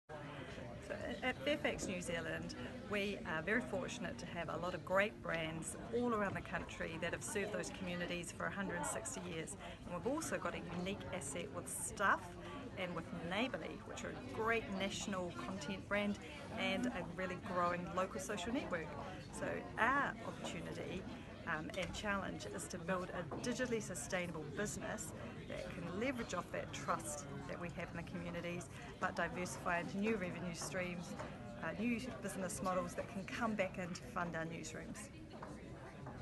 Fairfax Media New Zealand CEO Sinead Boucher discusses the importance of brand
INMA's Earl J. Wilkinson talks with Fairfax Media New Zealand CEO Sinead Boucher about the news media company's influential brands and how that is helping the company build a digitally sustainable business.